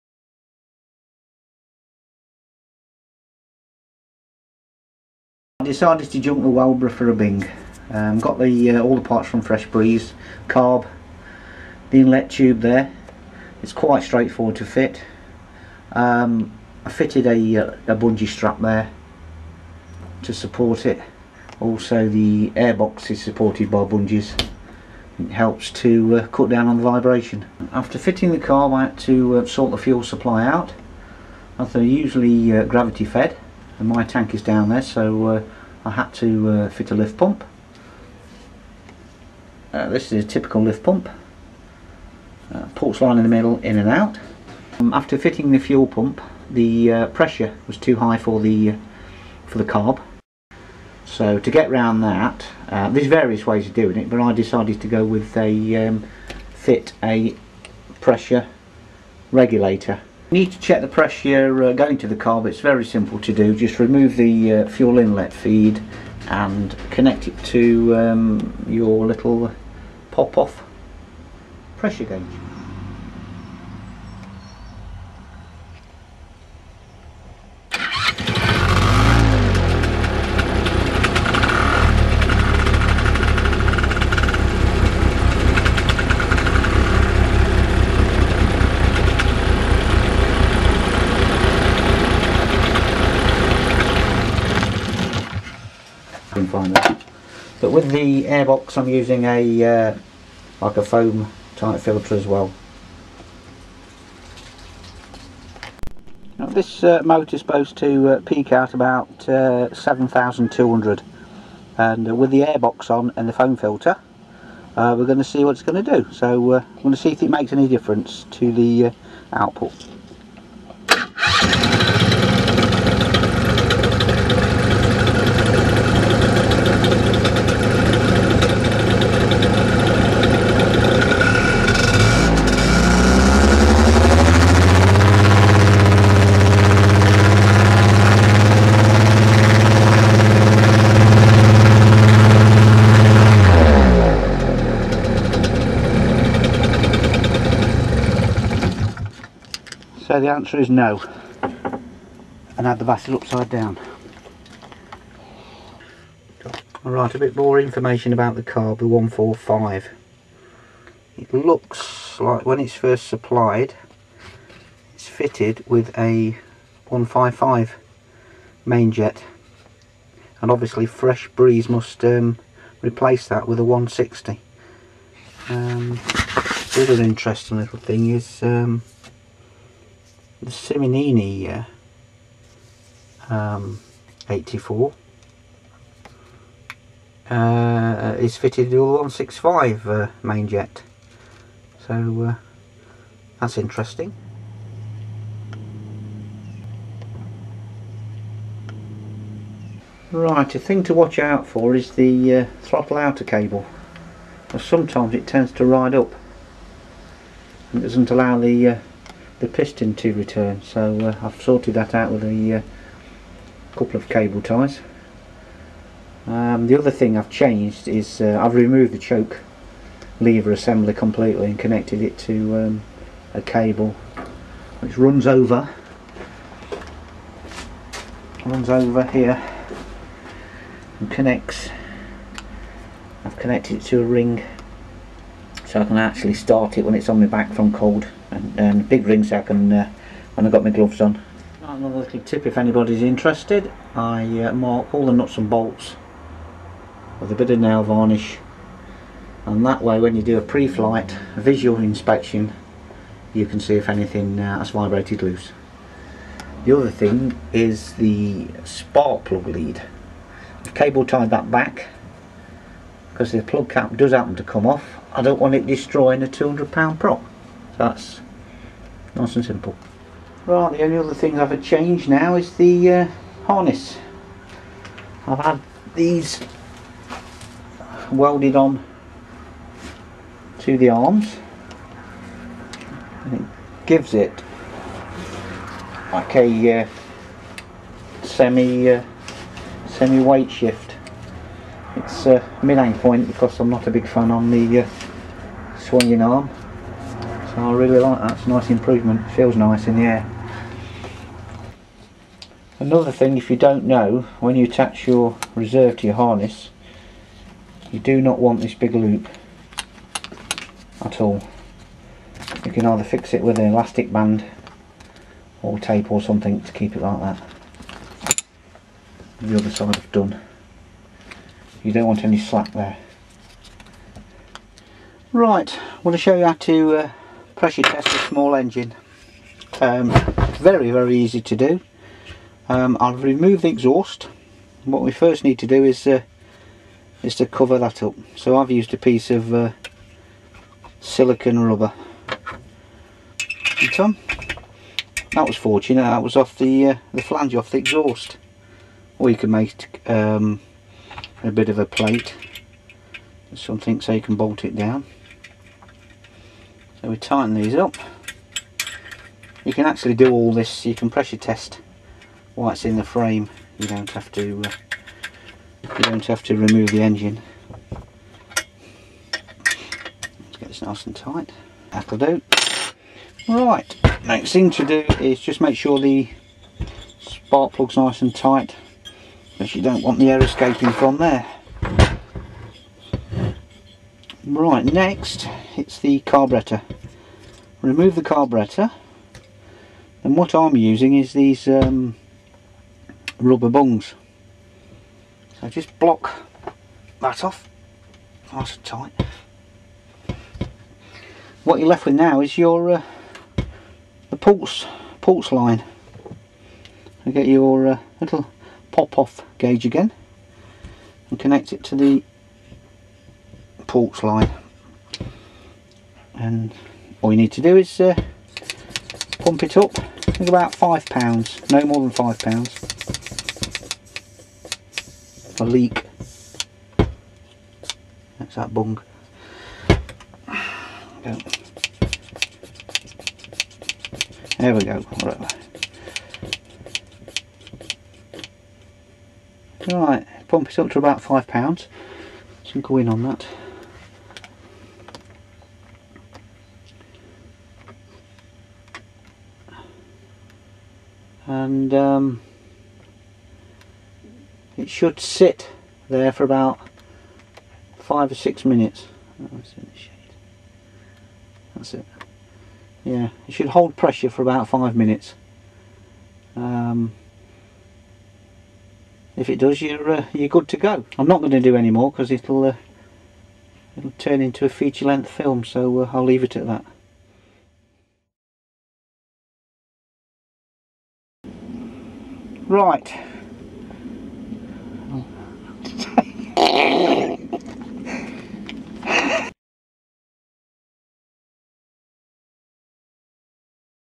2 stroke Pressure Testing. Bing carb paramotor
Changing over from a Walbro to a Bing carb, Fuel pump pressure, engine leak-down test.